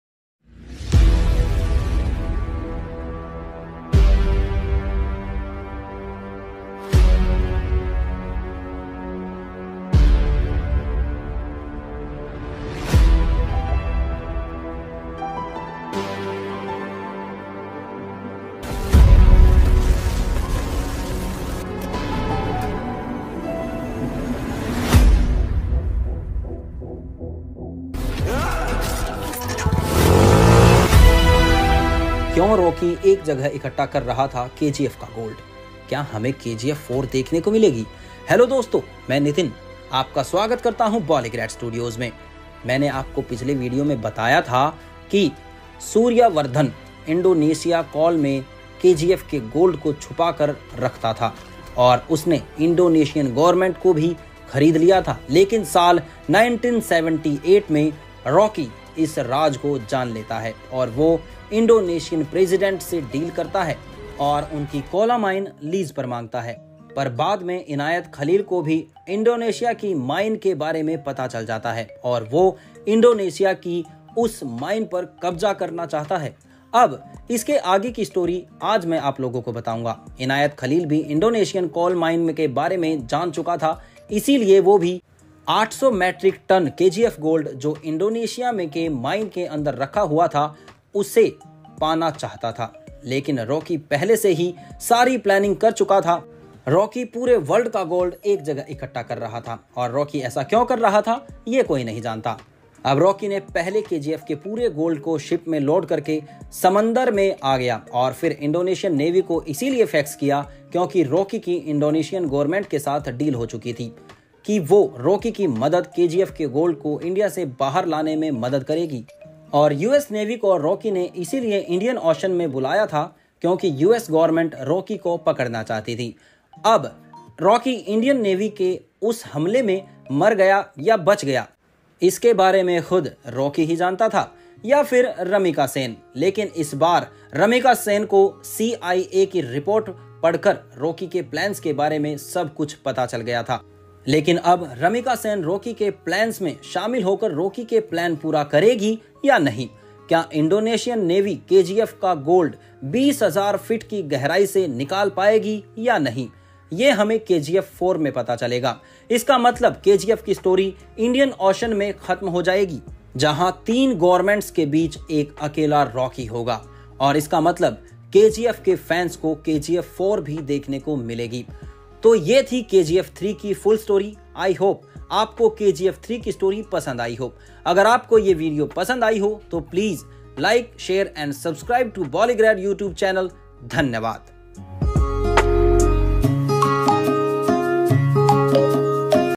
KGF 3 | 30 Interesting Facts | Yash | Srinidhi Shetty | Raveena Tandon | Prashanth Neel |Prakash Raj
‘KGF’ Director Says Part 3 Is Inevitable After Movie’s Success & We’re Celebrating Already

Key Questions=
0:00 Intro
0:31 kyun Rocky ek jagah ekkatah kar raha thha Kgf ka gol?
0:35 kya hame KGF 4 dekhne ko milegi ?

Director Prashanth Neel, globally known for his KGF films, recently opened up about the third part of the franchise, which has left the fans excited. Speaking to the YouTube channel Galatta Plus, the director said, “KGF Chapter 3 is possible. Cinema is the major force behind the success of KGF2. People have accepted the KGF world.

“Many have fallen in love with these cinematic characters. Thus, we want the story to continue. But when? We don’t know, but we are going to keep it going."

Neel also added that the third part has been in consideration for a long time but the team is now focusing on taking a big break after the blockbuster success of KGF Chapter 2.

Last month, Vijay Kiragandur, the producer of KGF, confirmed the project in an interview. He said that the third part in the franchise will go on floors later in October this year and it is expected to hit theatres in 2024.

Speaking of KGF Chapter 2, the film broke several records at the worldwide box office. Bankrolled by Vijay Kiragandur under the banner of Hombale Films, the film is a sequel to the 2018 movie, KGF: Chapter 1.

Bhuvan Gowda has handled the cinematography while Ravi Basrur has composed soundtracks for the film.

KGF: Chapter 2’s plot follows the story of Rocky (played by Yash), who after establishing himself as the kingpin of the Kolar Gold Fields, must retain his supremacy against enemies and government officials at the same time while coming to terms with his past.

In addition to Yash, the film has Srinidhi Shetty opposite the lead actor, playing the female lead role. It also stars Sanjay Dutt, Raveena Tandon, Prakash Raj, Malvika Avinash, Achyuth Kumar, Ayyappa P. Sharma, Rao Ramesh, and Eshwari Rao, among others in pivotal roles.

gd-abhi

Editor - Swastik Holkar
Anchor - Nitin Dahiya
Writer - Divanshu